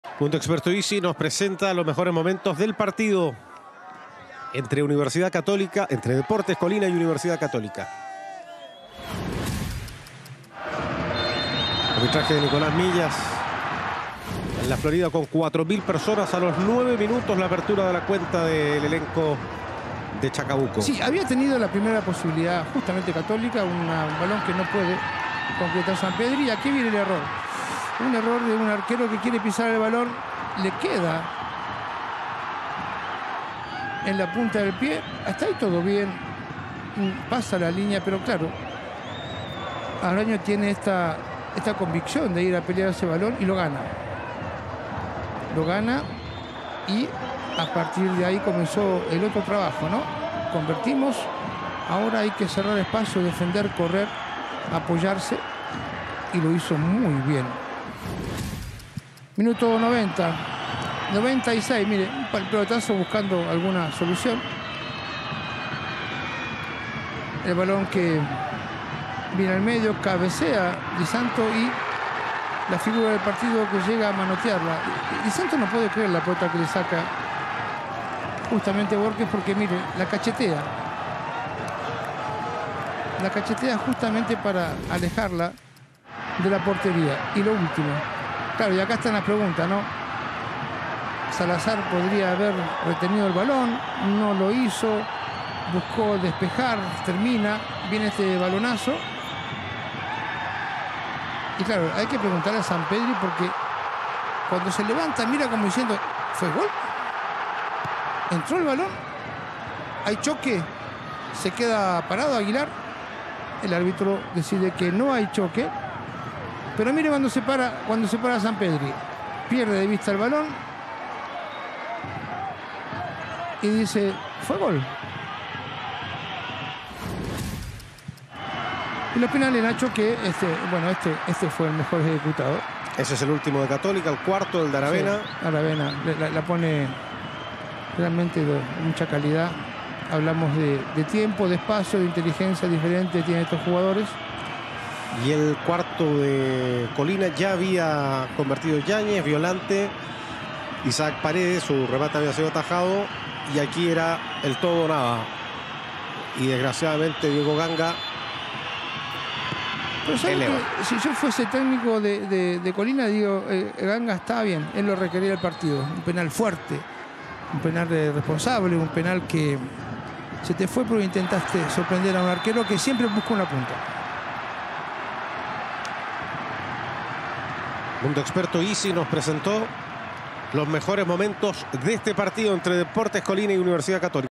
Dep. Colina (2) 1 - 1 (4) U. Católica | Copa Chile Easy 2023 - 8vos. de Final Zona Centro Norte
Los Cruzados sufrieron para superar a Deportes Colina en el Estadio Bicentenario de La Florida. En los noventa minutos, hubo paridad a un tanto.

Bastián Arraño aprovechó un error del guardameta Matías Dituro para adelantar a los locales y no fue sino hasta los últimos instantes del encuentro cuando apareció Fernando Zampedri para el 1-1. Todo se definió en penales y los dirigidos por Ariel Holan avanzaron.